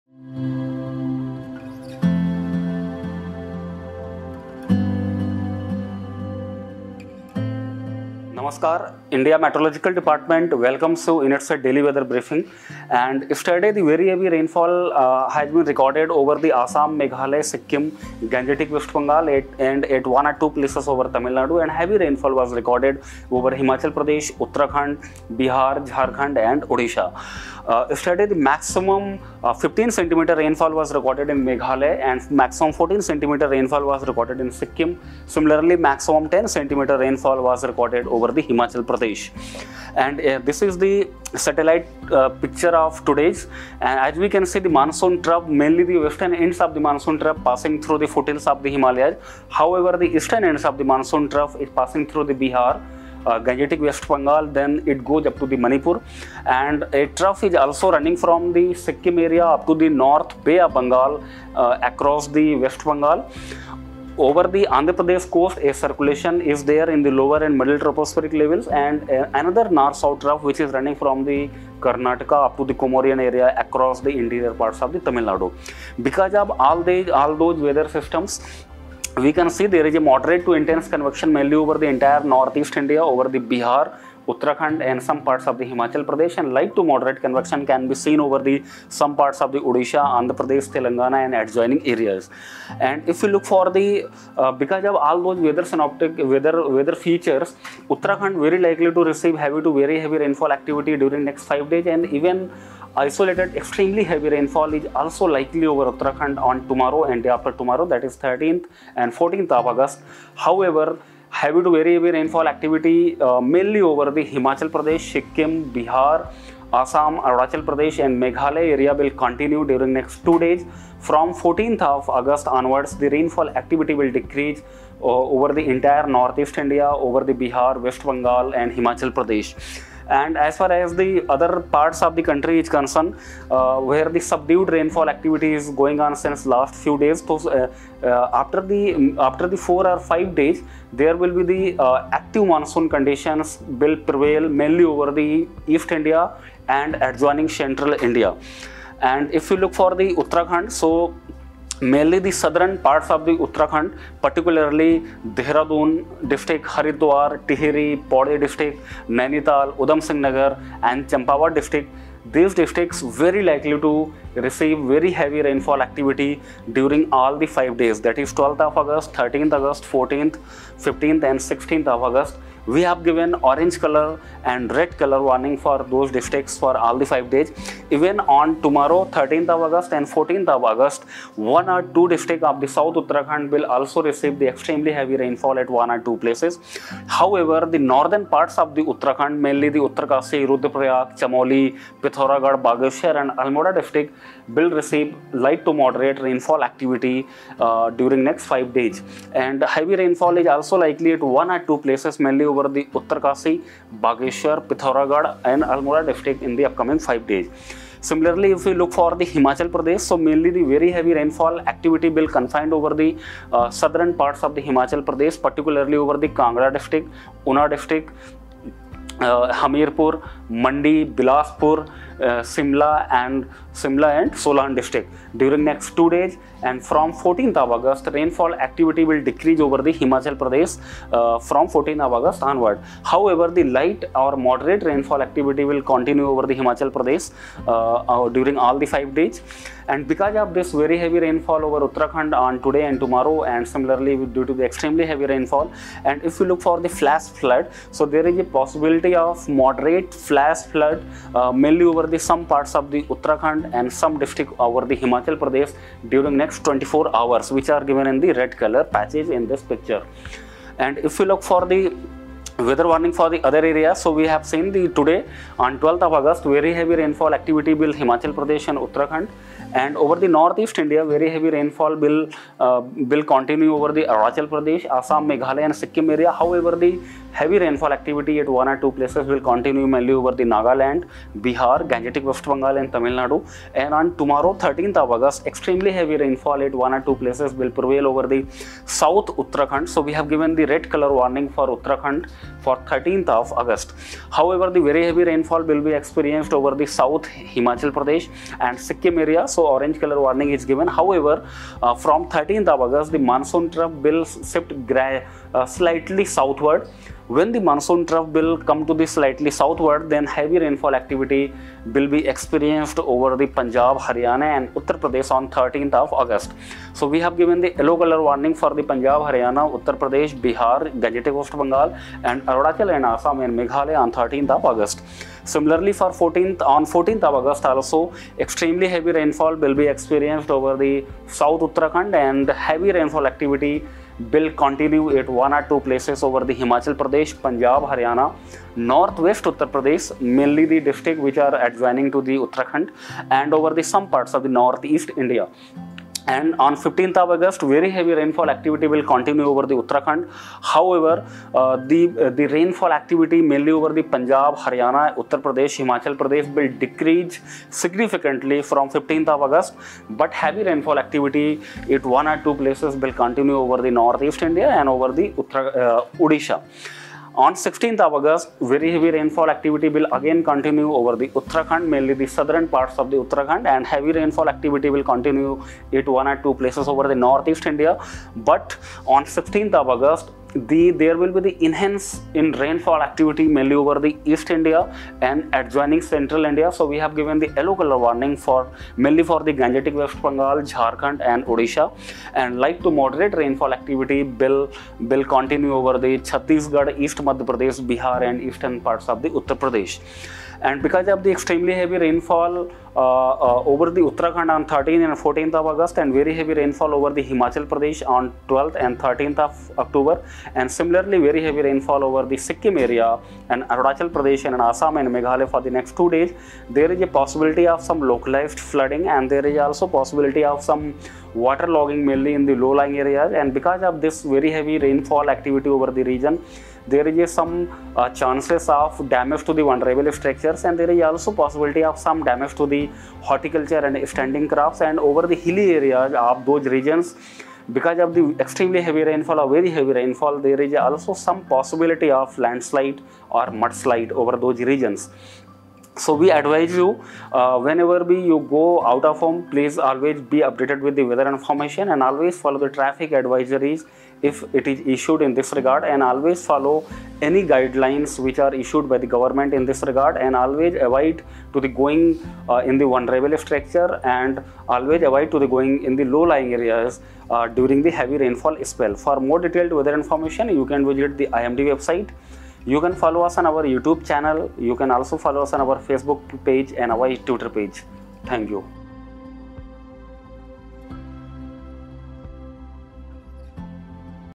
Heavy rainfall over Uttarakhand till 16th, over Himachal Pradesh & northeast India till 13th August.
Heavy to very heavy rainfall likely to continue over Uttarakhand during next five days and Himachal Pradesh, Bihar, Sub Himalayan West Bengal, Sikkim during next two days with extremely heavy falls over Uttarakhand on 13th & 14th August.

Isolated heavy to very heavy rainfall likely to continue over Arunachal Pradesh and Assam & Meghalaya on 12th and decrease thereafter.

Subdued rainfall activity likely over rest parts of the country during next 4-5 days and likely revival of active Monsoon conditions thereafter.